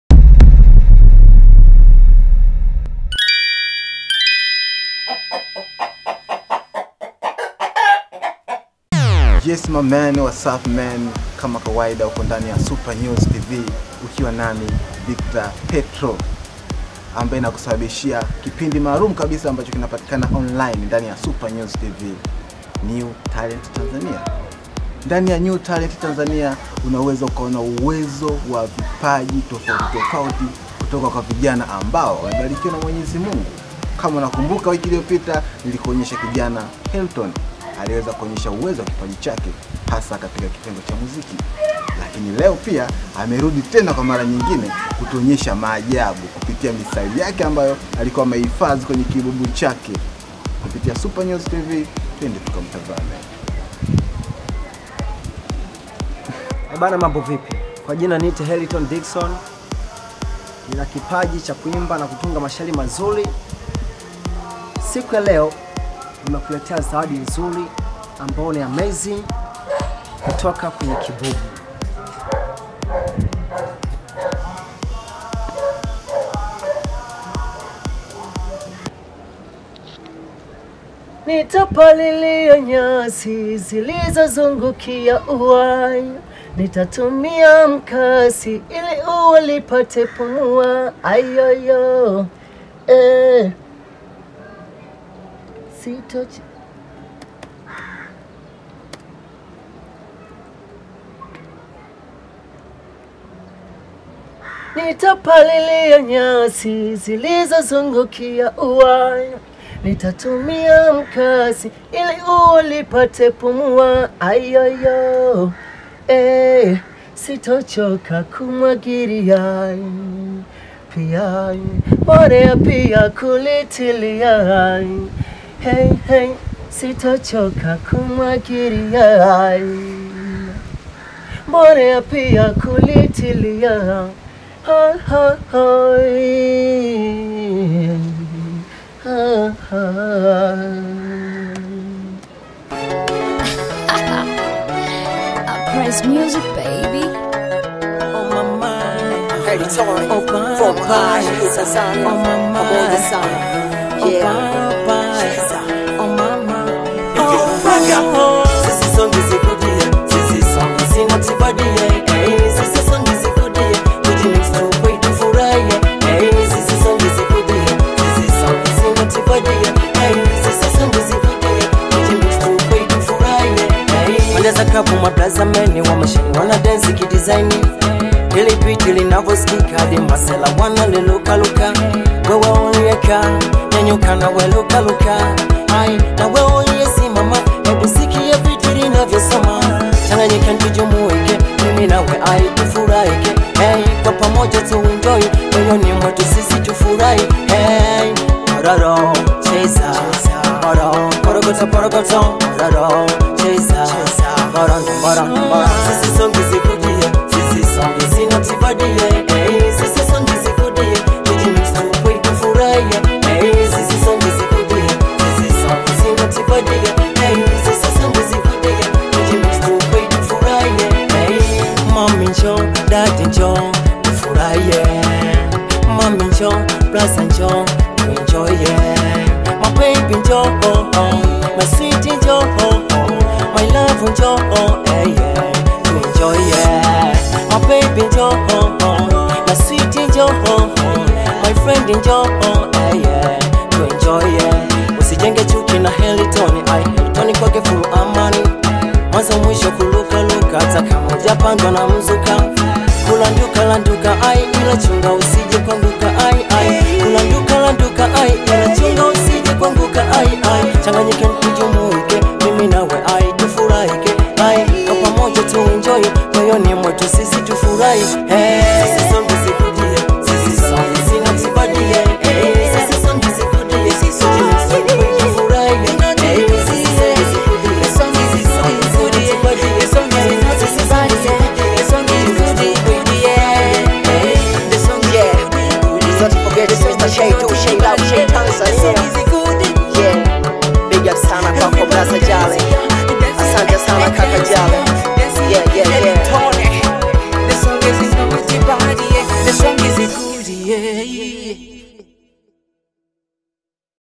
VIDEO: Msanii Helton atoa zawadi kutoka kwenye kibubu cha miaka 6!
Msanii wa Muziki wa Bongo Fleva ambaye anatambulika kama Helton Dickson ameachioa zawadi ya misitari michache kutoka kwenye kibubu chake cha ngoma kilichao anza kuhifadhiwa mashairi miaka 6 Sita Iliyopita. Msanii huyo akiwa ndani ya Super News TV alizungumza machache na kutoa zawadi ikiwa imeambatana Ngoma yake ya Kwanza.